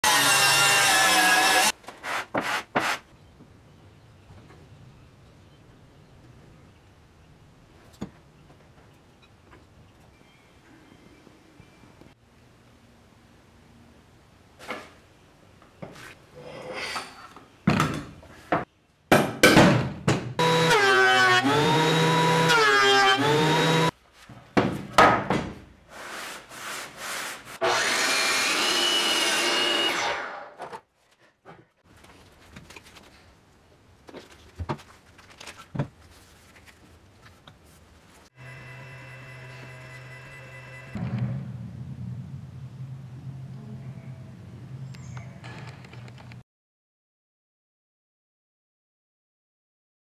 Making a teak cutting board.
A quick short about making a cutting board out of teak, using basic woodworking tools. Ripping the teak into strips, sanding them to remove saw marks, arranging, glue up and clamping, planing, sanding, oiling and finishing.